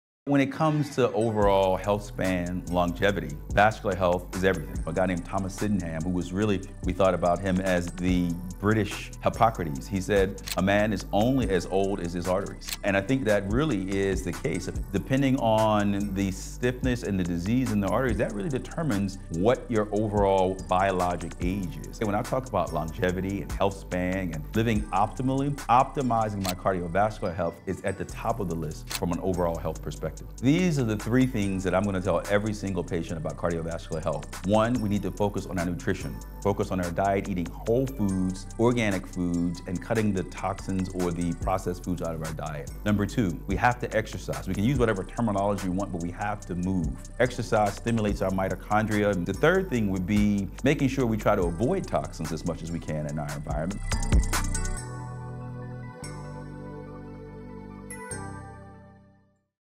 Vascular Health: The Real Key to Longevity! 3 tips to improve cardiovascular health
Want to live a long, healthy life? Prioritize your vascular health! 💯

As the "British Hippocrates" Thomas Sydenham said, "A man is only as old as his arteries." Arterial stiffness & disease directly impact your biological age. Optimizing cardiovascular health is KEY for longevity and healthspan. Here are the 3 tips for a better cardiovascular health:

- Nutrition: Fuel your body with whole, organic foods and ditch the processed junk.

- Exercise: Move your body! Exercise boosts your mitochondria, the powerhouses of your cells.

- Minimize toxins: Reduce your exposure to environmental toxins because they cause inflammation and oxidative stress in our bodies.

Take care of your vascular health, and you'll take care of your whole body!